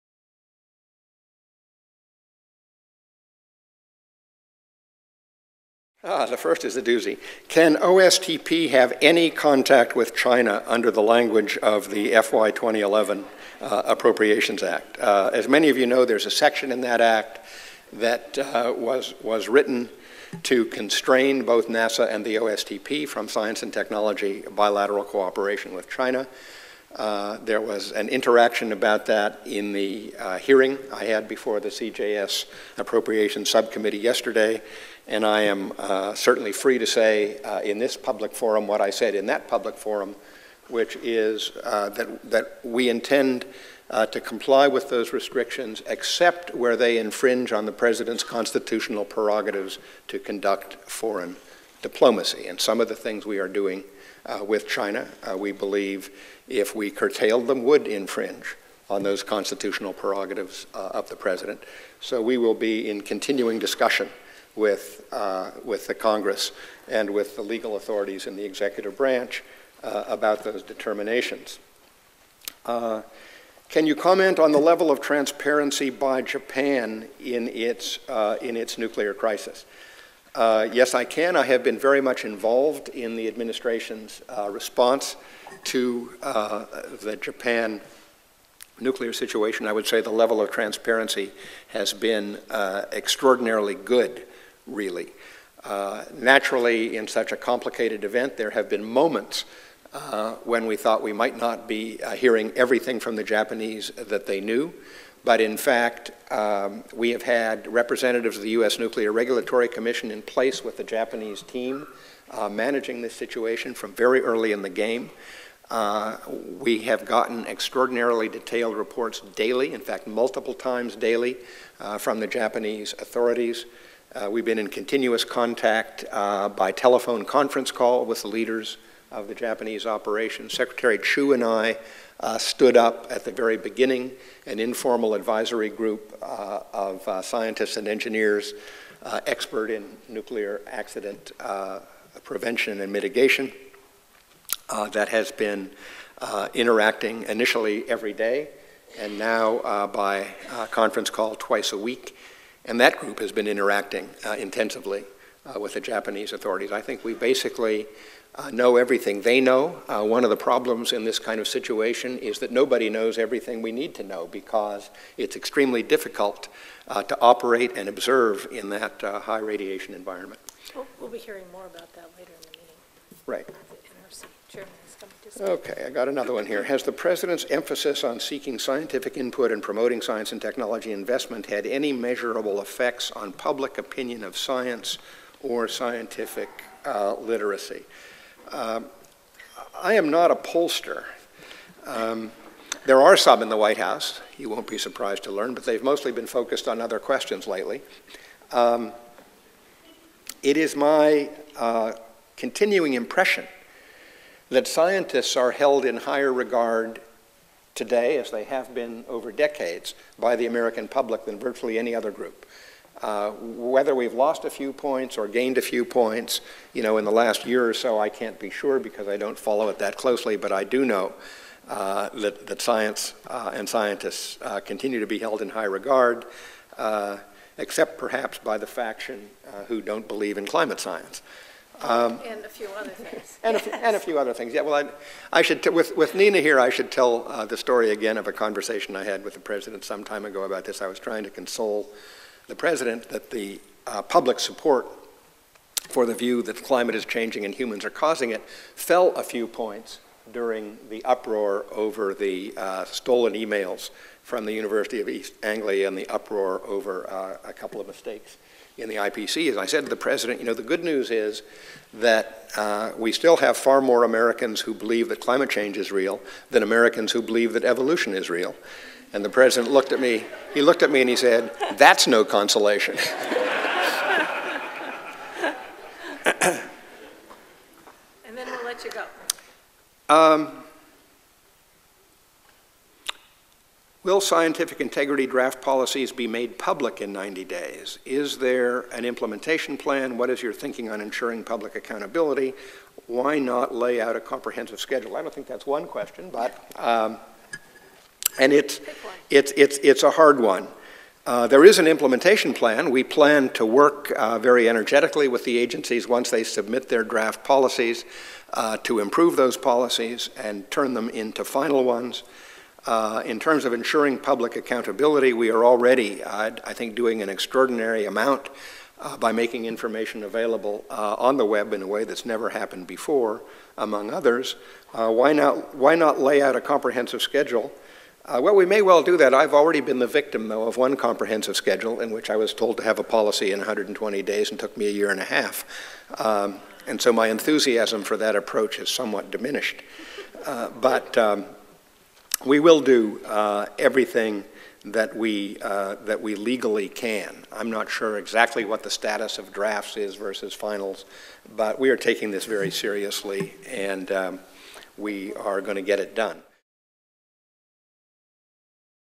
John P. Holdren - Q&A
Questions and Answers segment- John P. Holdren

2011 AAAS Forum on Science and Technology Policy
5 May 2011
Washington, D.C.